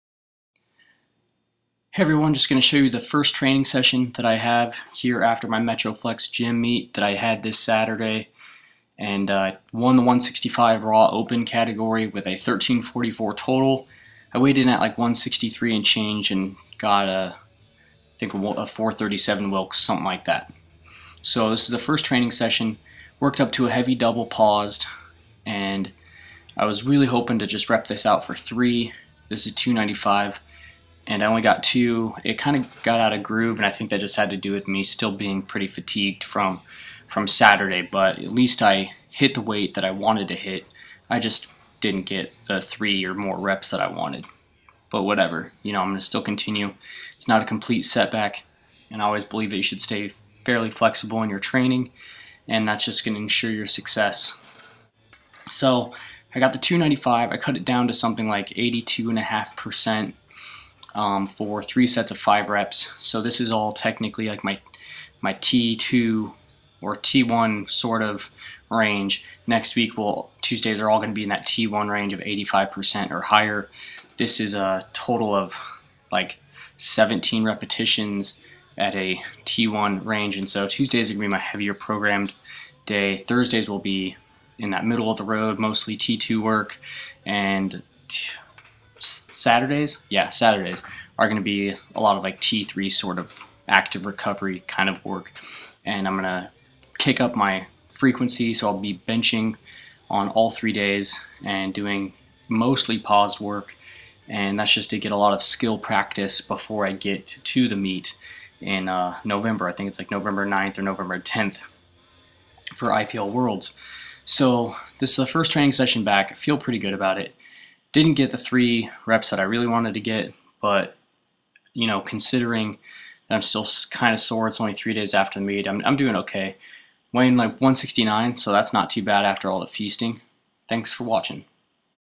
IPL Worlds Prep Bench #1
Worked up to a 295 lb. rep out and didn't get the reps I wanted. Still a bit fatigued from last Saturday's meet. All in all, a good training session. Nice and short. It's good to be back.